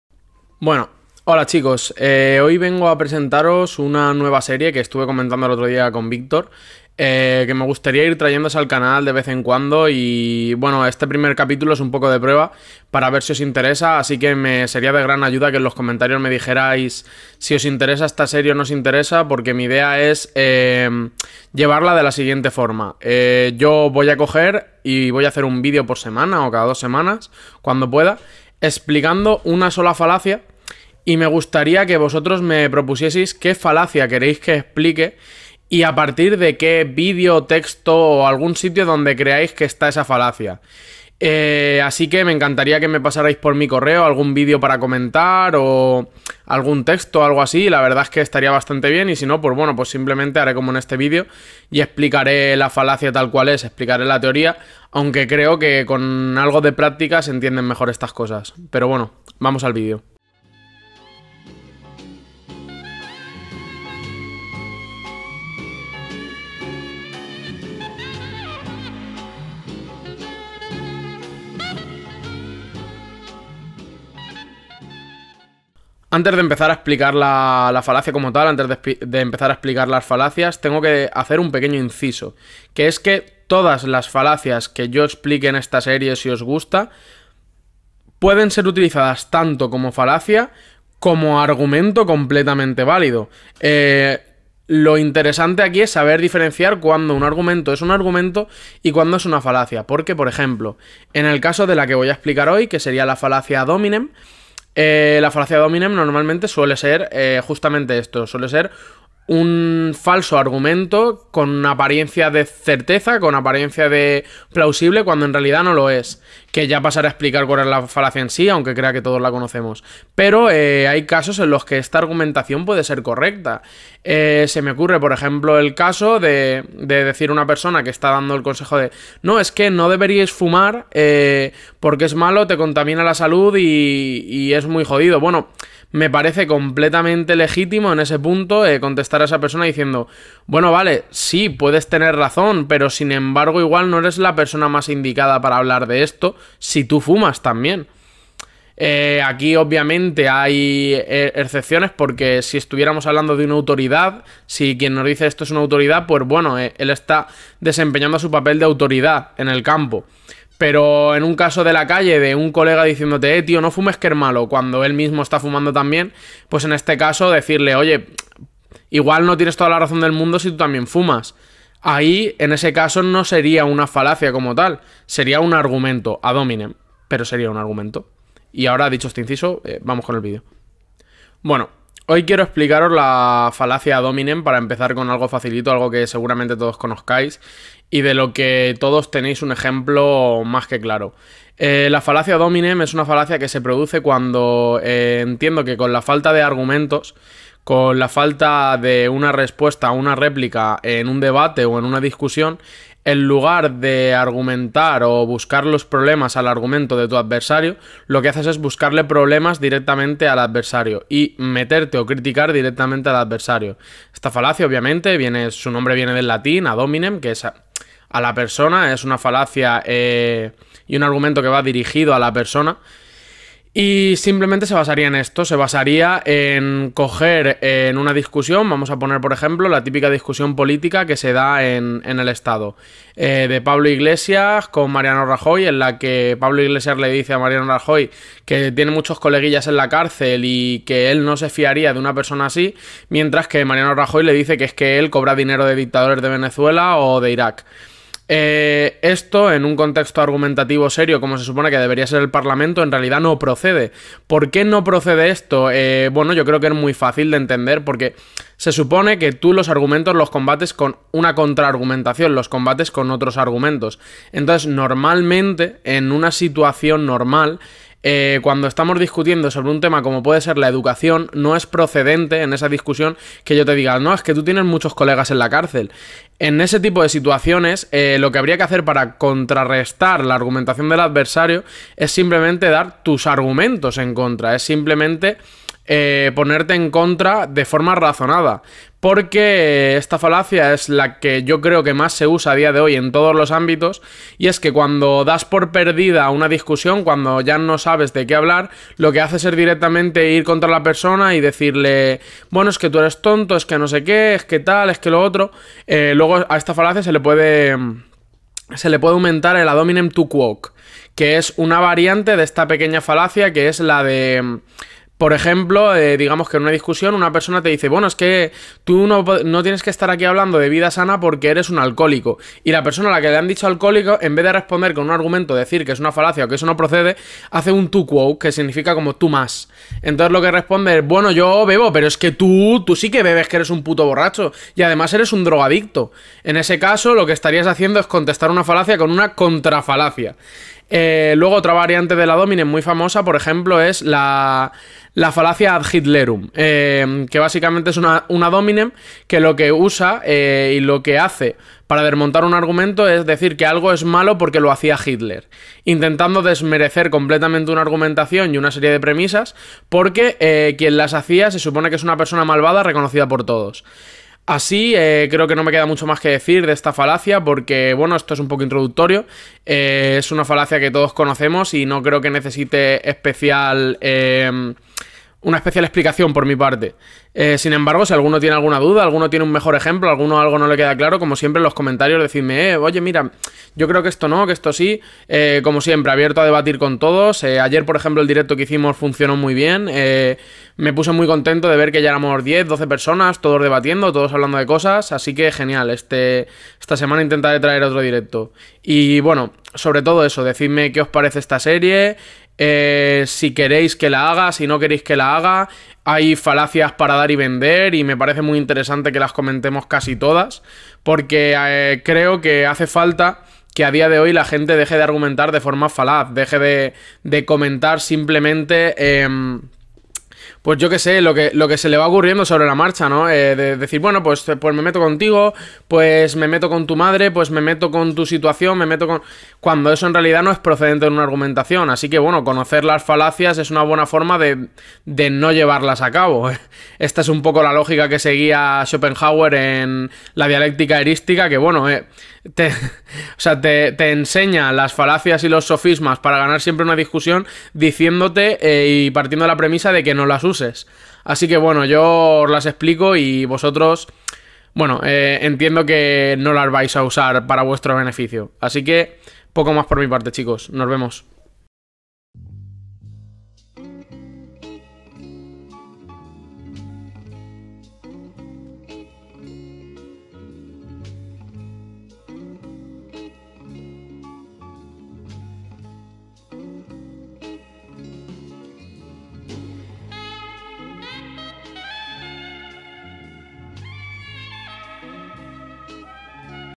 FALACIA AD HOMINEM
Buenas a chic@s en el vídeo de hoy empiezo lo que me gustaría que fuese una serie en la que os voy a explicar una a una las falacias más comunes usadas por todo el mundo. También me encantaría que me ayudaseis pasándome vídeos donde encontréis falacias ineresantes de comentar.